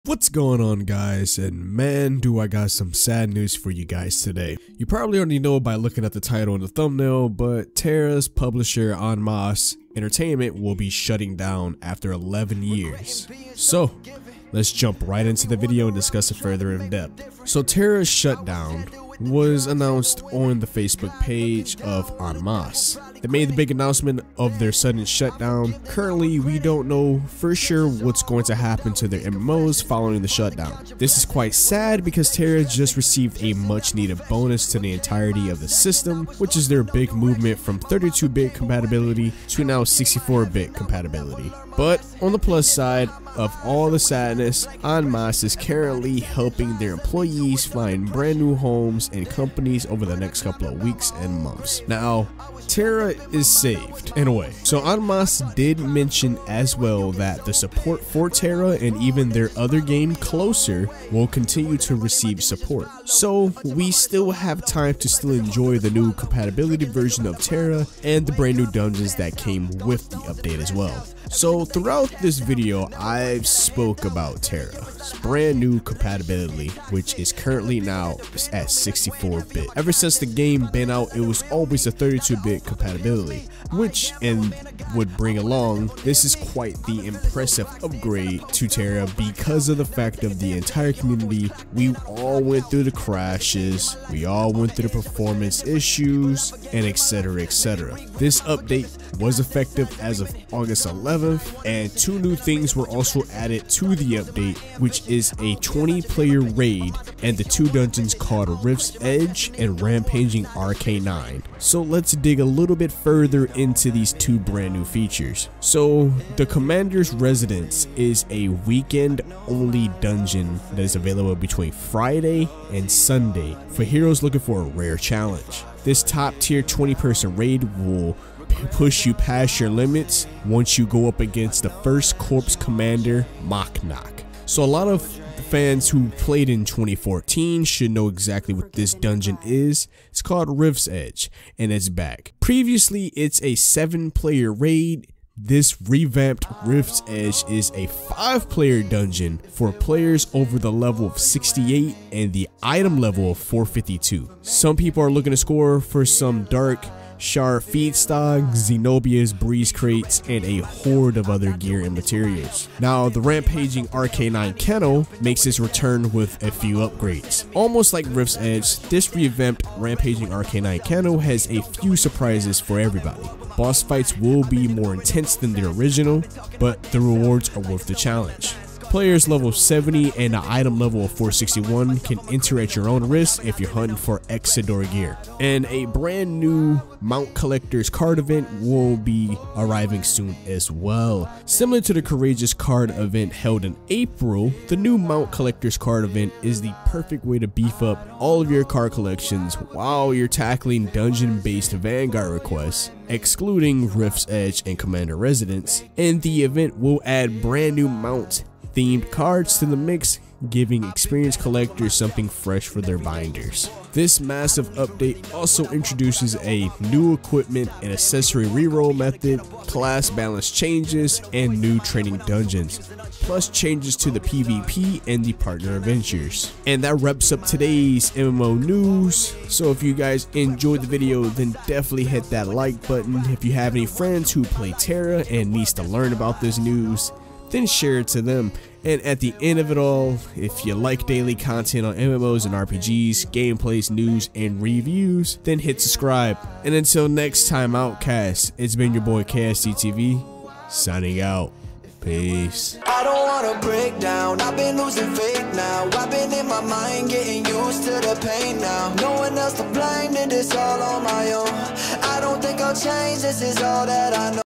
Is Tera Online getting SHUT DOWN?! + Tera is now 64-Bit Compatible | Tera Online 2020
Is Tera Online getting shut down?! its actually En Masse the publisher for Tera is getting shut down but they did say we will get constant support for Tera Online so the game still lives but more information will be released at a later date. As this is all happening Tera Online just received a major boost from changing there 32 bit compatibility to now its 64 bit compatibility. The game runs a lot smooother and the performance is so much better

Tera Shut down 0:15